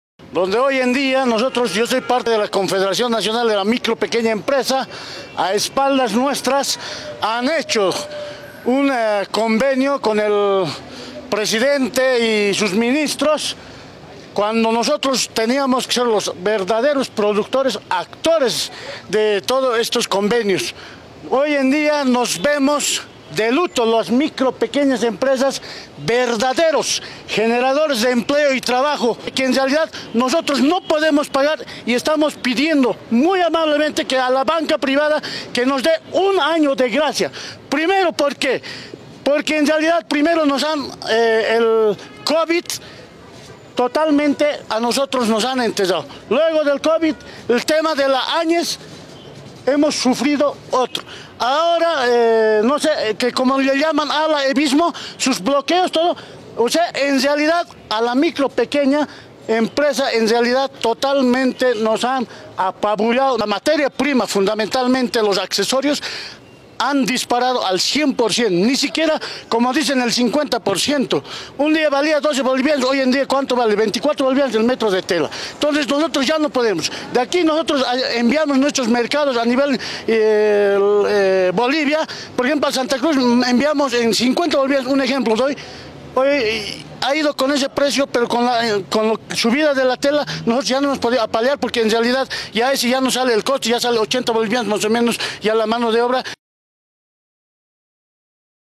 Microempresarios de Bolivia piden al gobierno que instruya a la banca privada otorgue un año de g
Microempresarios de Bolivia piden al gobierno  que  instruya a la banca privada otorgue un año de gracia a sus créditos, ante la crisis económica en el sector.

El Representante de la Confederación de la Micro y Pequeña Empresa Eliberto Wilmer  Yanque Mita.  Dijo que su sector no fue convocado por el gobierno nacional a ser parte del Diálogo Nacional por la Economía y la Producción para ser atendidas  sus demandas, como son el incremento de la importación de la materia prima e insumos por la escasez del dólar  que está provocando el cierre de unidades productivas. Por lo que la dirigencia del sector pidió al gobierno nacional a que autorice a la banca privada a que les otorguen un año de gracia a sus créditos que tienen con las entidades financieras  ya que sebe  imposibilitados de pagar sus créditos.